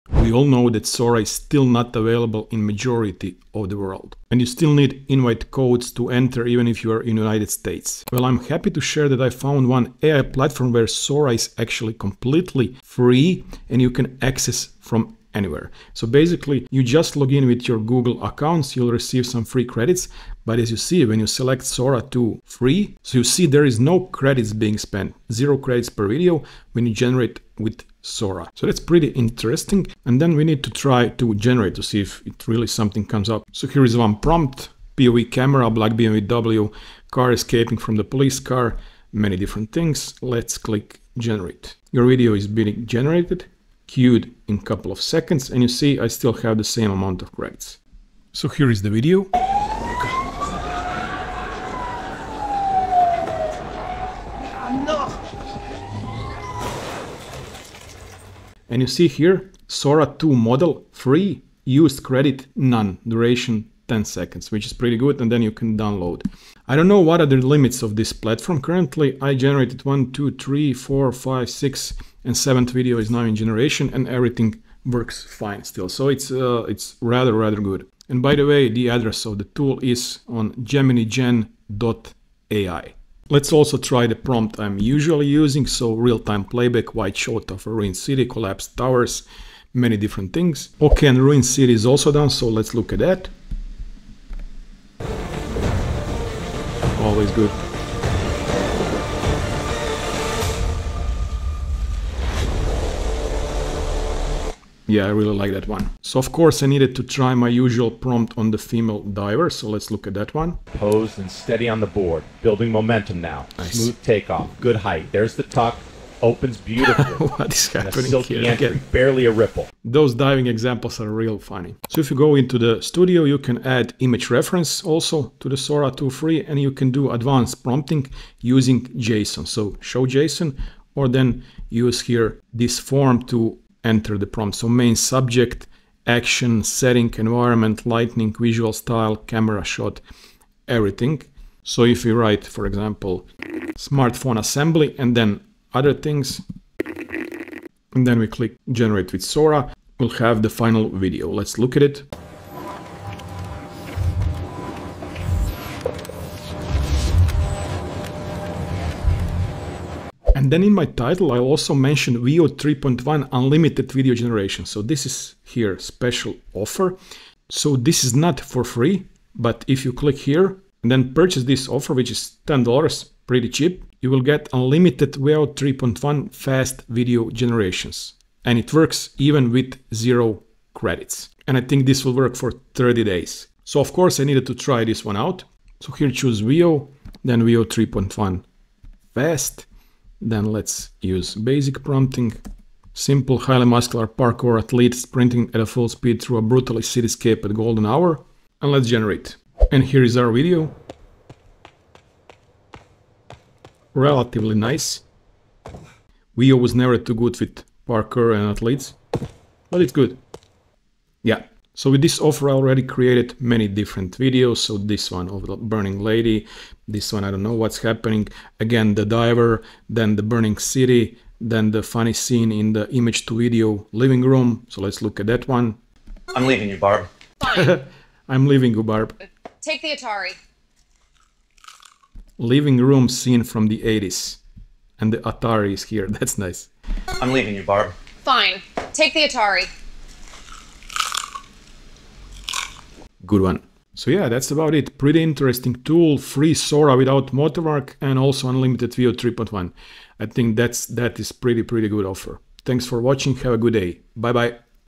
Free Sora 2 Without Watermark + Unlimited Veo 3.1! (Full Tutorial)
Free Sora 2 Without Watermark is finally here! In this video, I’ll show you how to use Sora 2 for free from anywhere in the world and without invite codes. You’ll also see how to use Veo 3.1 for unlimited generations.

With this AI platform, you can create Sora 2 videos for free, export them without any watermark, and unlock Veo 3.1 unlimited video generation for cinematic, professional-looking results.

I walk through everything — from logging in and testing Sora 2 free credits, to creating realistic prompts, using JSON for advanced control, and accessing Veo 3.1’s unlimited mode.

• How to access Free Sora 2 Without Watermark from anywhere
• Step-by-step setup to generate videos with zero credits
• How to use Sora 2 JSON mode for advanced prompting
• Full walkthrough of Unlimited Veo 3.1 video generations
• Example clips — cars, divers, city ruins, and cinematic scenes